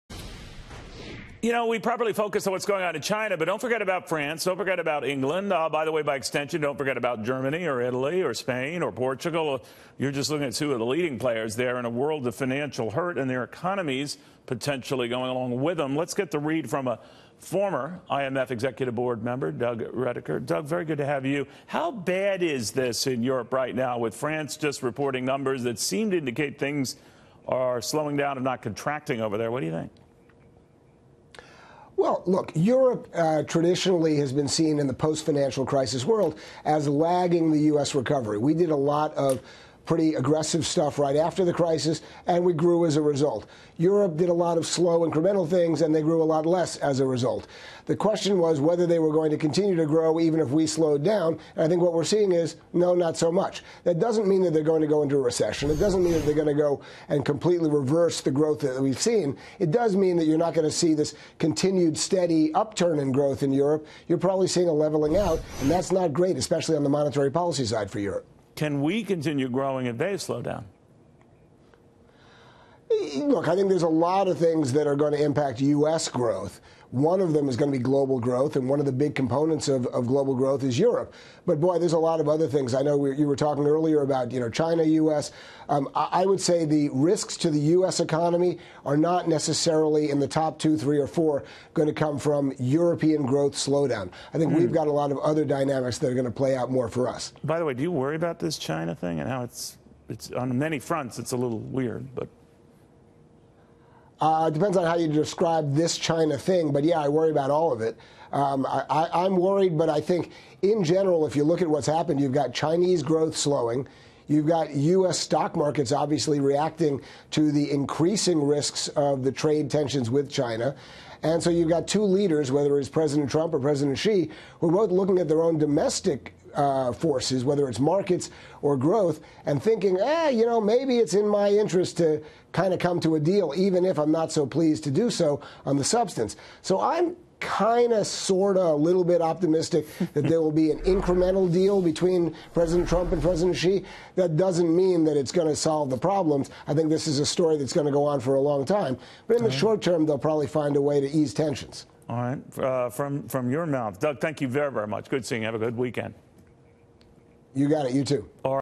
European economy under pressure
Former IMF Executive Board member Doug Rediker on the European economic concerns.